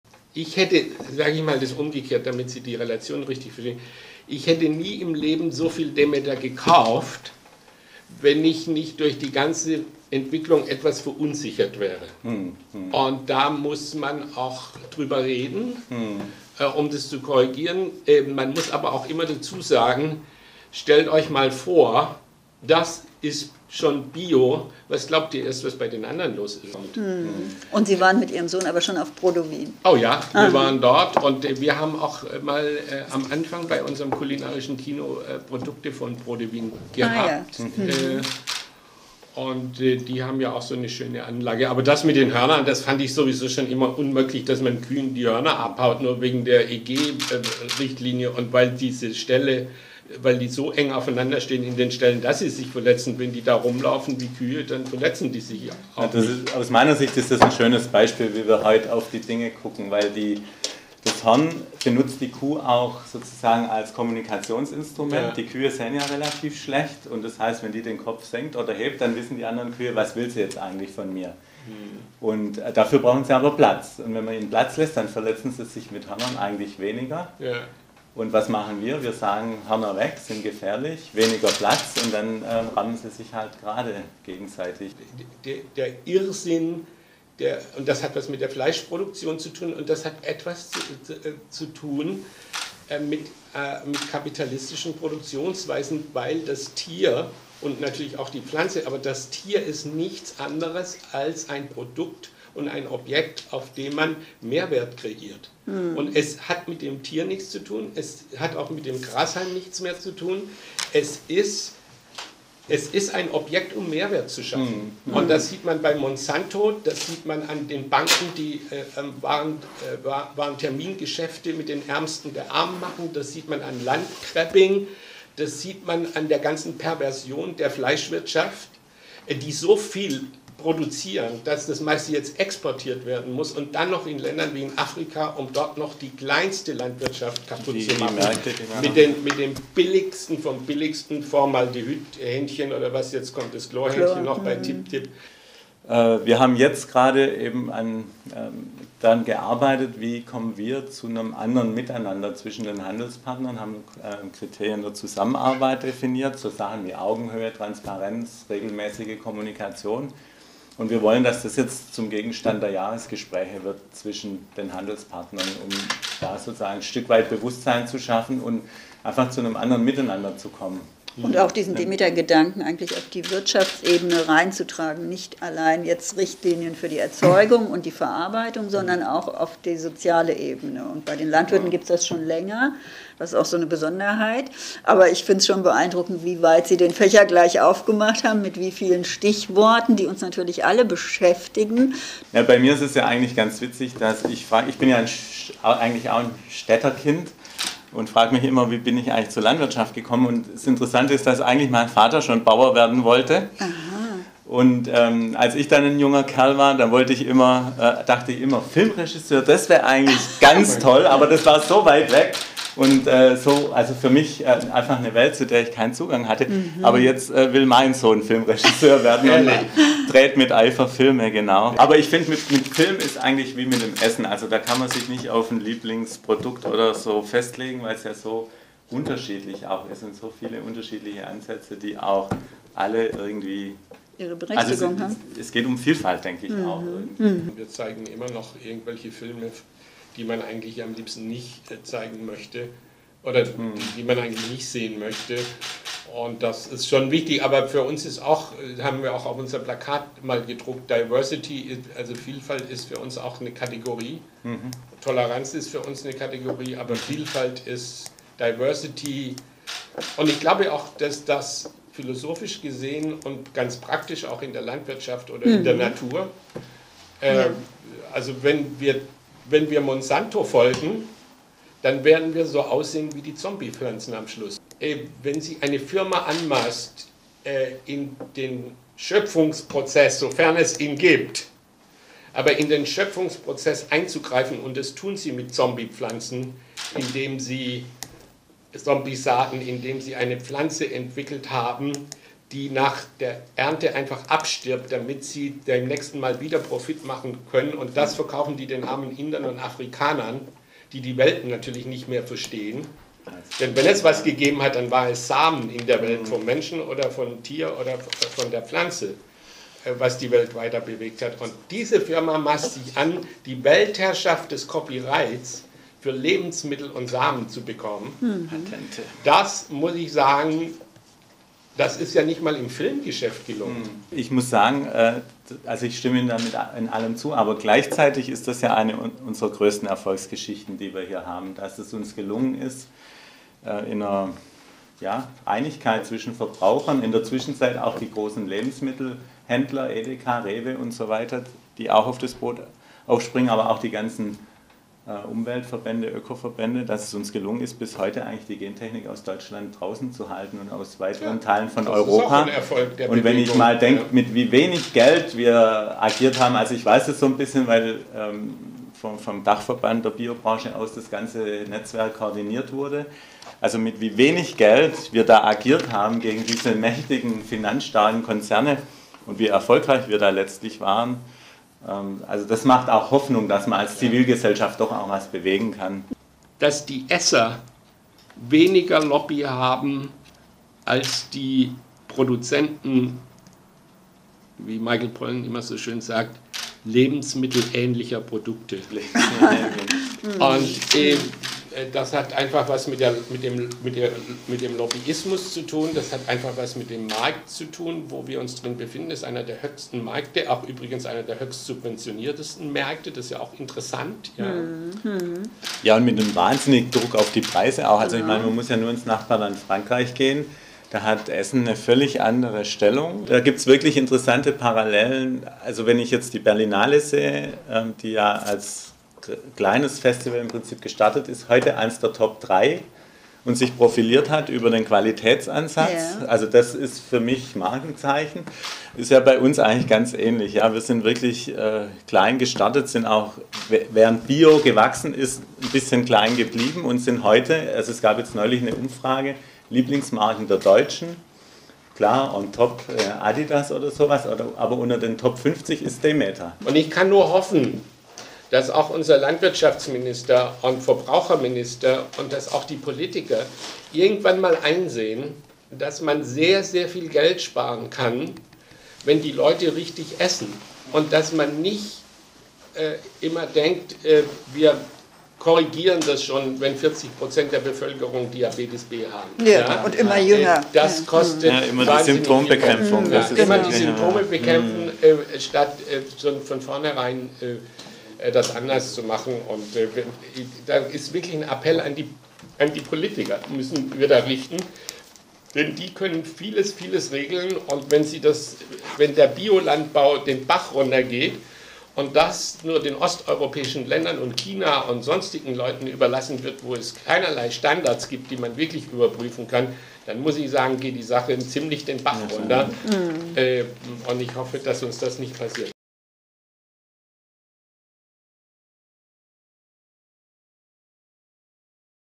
Demeter-Vorstand Alexander Gerber im Gespräch mit Berlinale-Direktor Dieter Kosslick
Zu Beginn des Gesprächs schenkt Demeter-Vorstand Alexander Gerber Berlinale-Direktor Dieter Kosslick ein Kuhhorn mit Zapfen und erklärt, warum Hörner so wichtig für die Kuh sind.

Das Gespräch über Hörner, Kühe und Filme moderiert Demeter-Pressesprecherin Renée Herrnkind. Sie finden es auch im aktuellen Demeter Journal